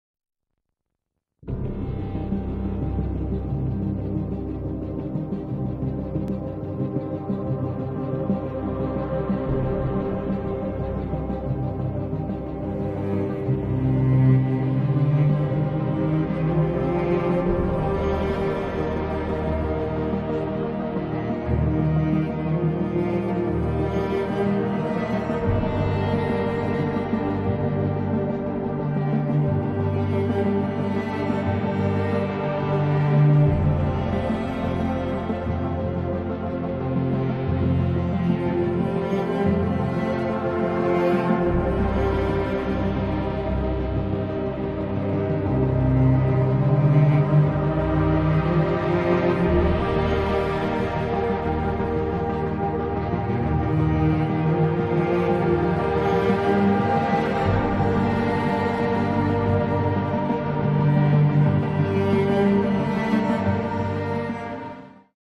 TATABÁNYA RÉGI BUSZÁLOMÁSA
Created by VideoShow IRATKOZ FEL 2021 TATABÁNYA RÉGI BUSZÁLOMÁSA 2006 IG ÜZEMELT utána lett az új vértes center 2007 ben az új buszálomás régit ott hagyták romosan és szemétel meglehet tekinteni a régi buszálomást mára nagyon rossz álopotba van 😪